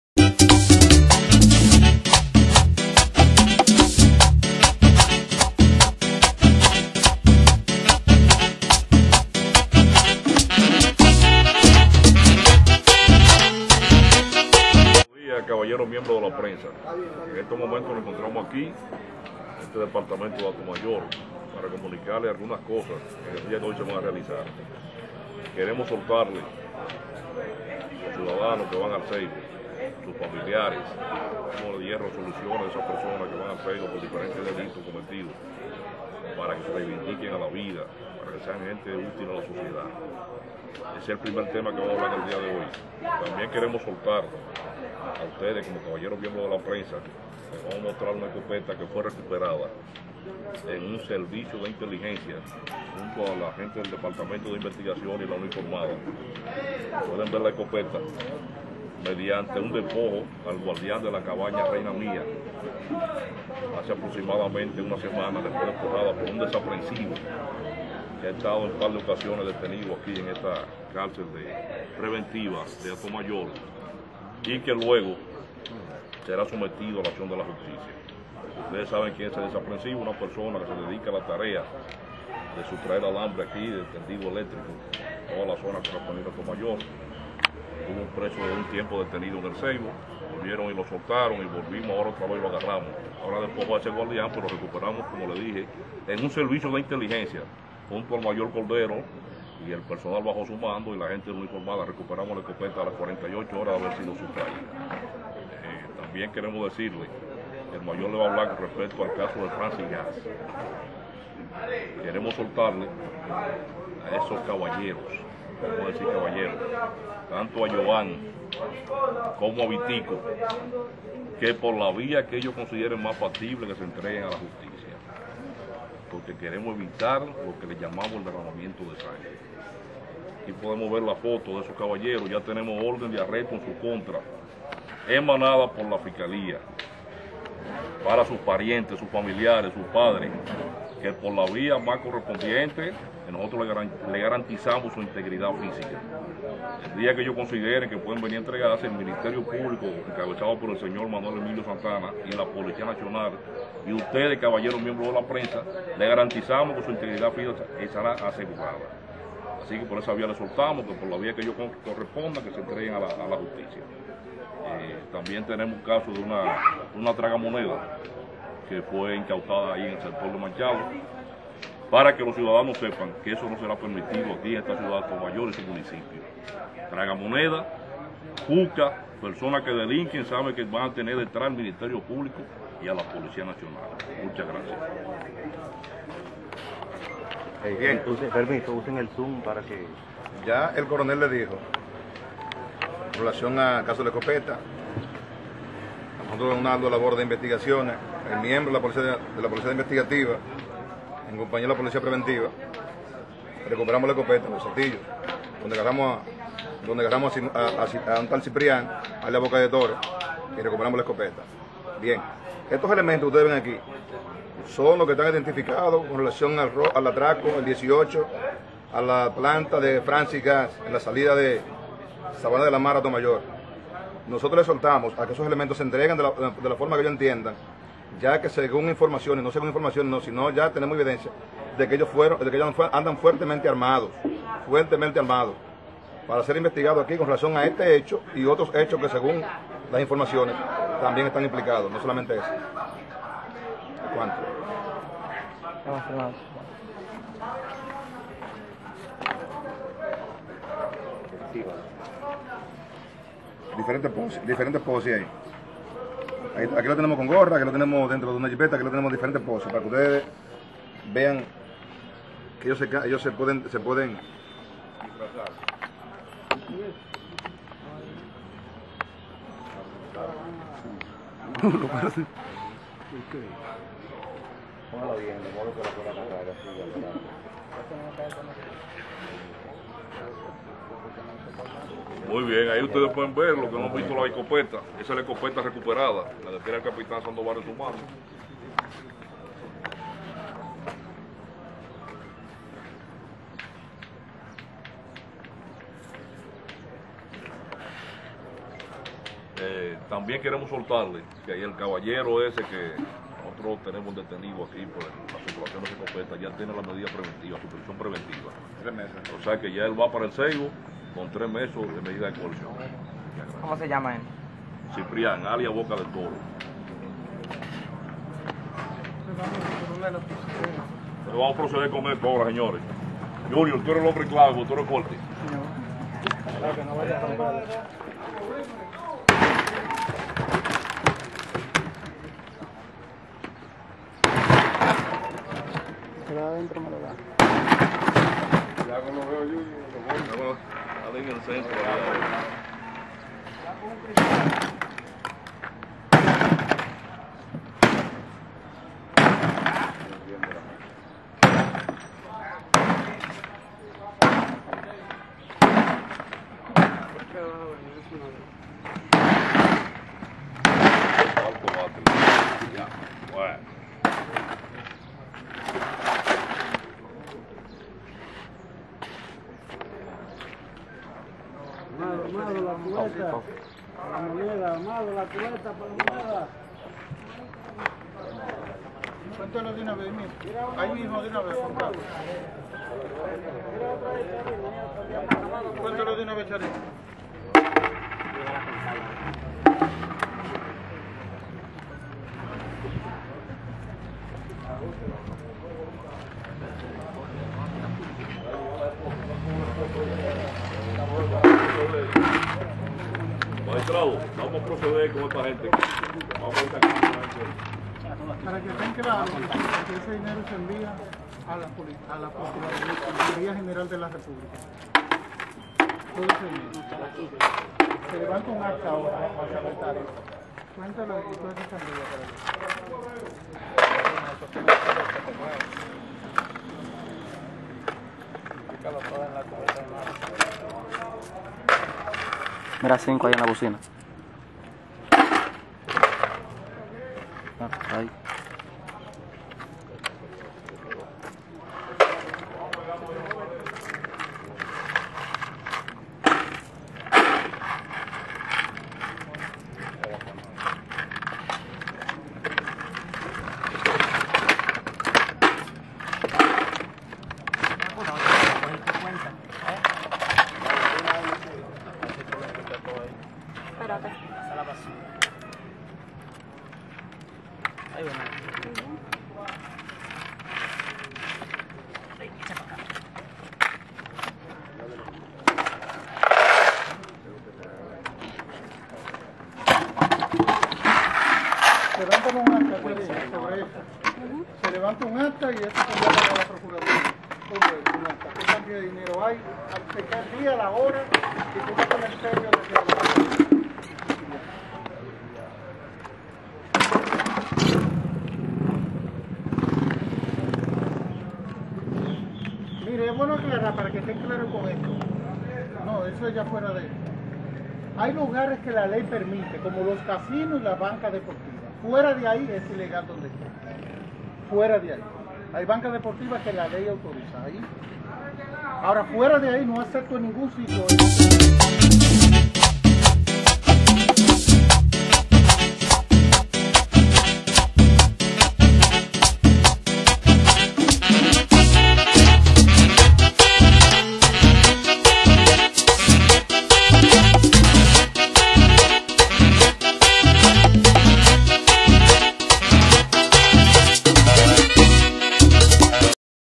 Autoridades buscan profugos, desmantelan maquinitas tragamonedas
Autoridades del Ministerio Publico y la Policía Nacional de Hato Mayor del Rey, presentaron a la opinión publica a los ciudadanos Víctor Manuel García, alias ''Vítico'' y Yoan Carela Benítez, alias ''Yoan'', quienes según los informes estos fueron los autores del atraco a mano armada a la bomba de Francis Gas ubicada en la carretera Hato Mayor - Sabana de la Mar. También las autoridades presentaron una escopeta recuperada la cual fue despojada al vigilante de una cabaña en esta ciudad y destrozaron maquinas tragamonedas incautadas en Colmadones y Cafeterías.